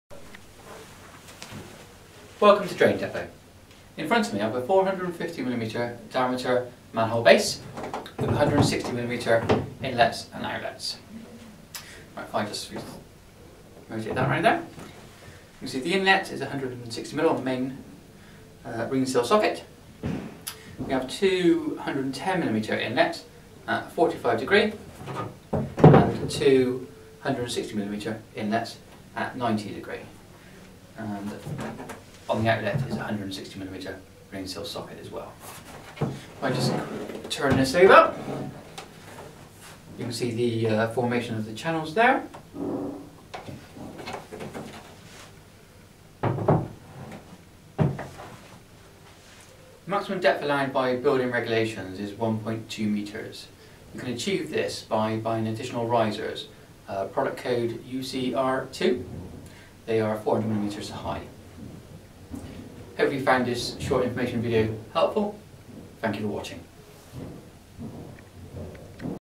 160mm PVC Underground Drainage 450mm Circular Chamber Base
450mm internal diameter polypropylene manhole base with 6 number ring seal sockets that accept 160mm & 110mm drain pipe or fittings. 3 number 160mm inlets, 2 number 110mm inlets and 1 number 160mm outlet. Complete with 3 socket plugs for blanking of any un-used inlets. Overall height is 400mm. Maximum depth allow by building regulations is 1.2 meters.